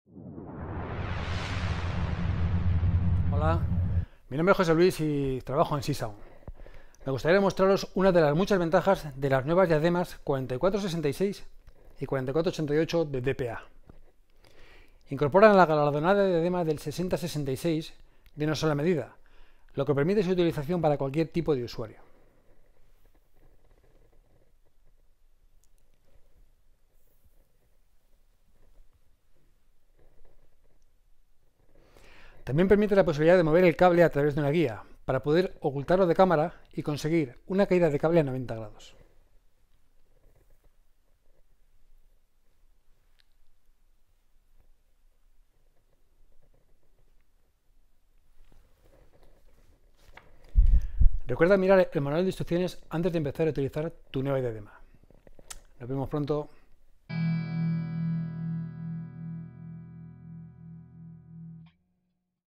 Micrófono de Diadema (headset) DPA modelo 4466 y 4488
Y hoy le toca el turno a nuestro compañero José Luis, que nos enseña otra ventaja mas de los nuevos micrófonos 4466 & 4488 de DPA.

Grabado con un iPhone XR un 4488 y un MMA-A

Ventaja 3: Incorpora la galardonada diadema del 6066, de una sola medida, permitiendo su colocación en todo tipo de usuarios y ademas permite la posibilidad de mover el cable a través de una guía hacia cualquier lado, permitiendo así poder ocultarlo de la cámara y una caída del cable a 90º